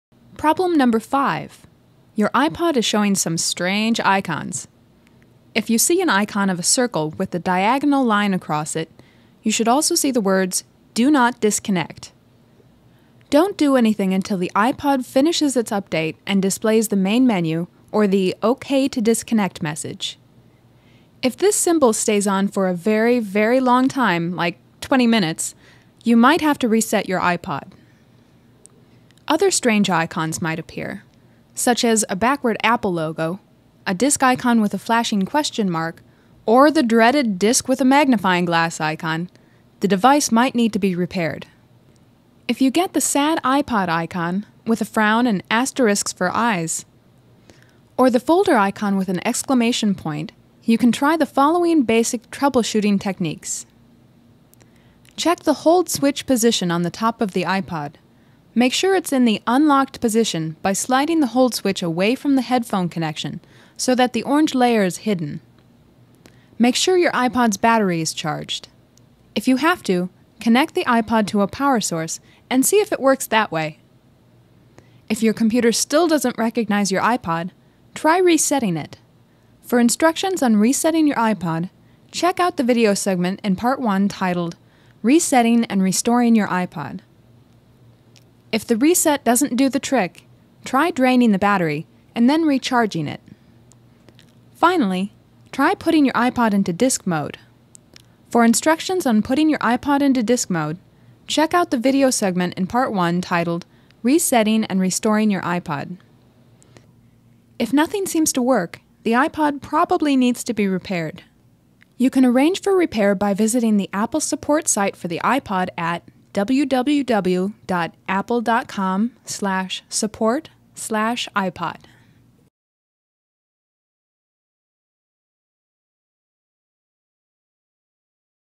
Understand Your iPod's Warning Icons For Dummies
When your iPod is having trouble, different icons may show up to symbolize different problems. See what to do if any of the warning icons show up unexpectedly on your iPod's display.